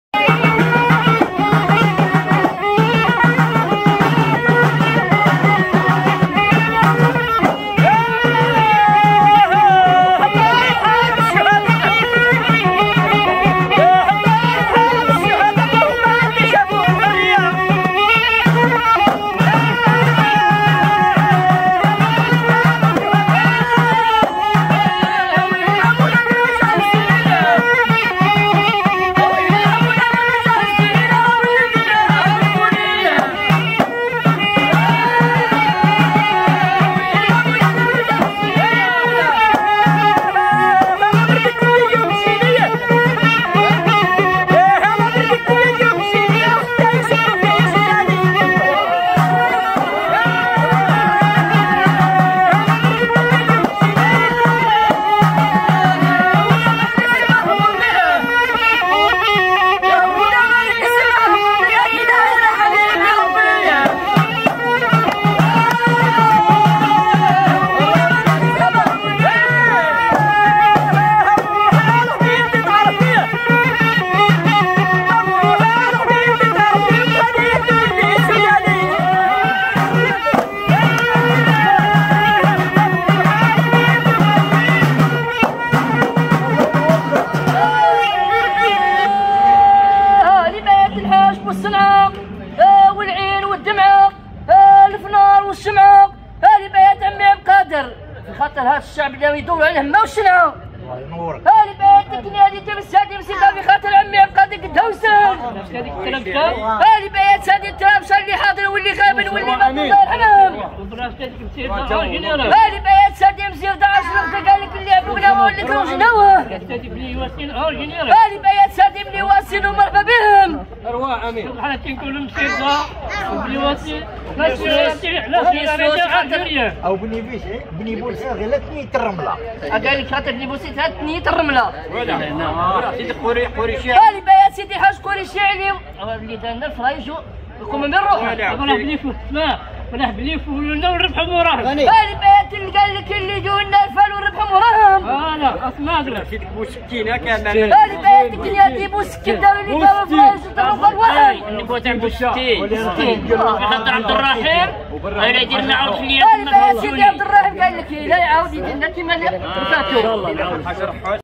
AARFA MSIRDA ksidate trés fort fikhatar chikh benyounes maéstro nta3 al3arfa 💪💪💪
mariage à maghnia salle alizza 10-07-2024
chikh benyounes chikh miloud chikh bilal chikh Tayeb chikh mohammed chikh nassim chikh Kamel chikh abdannour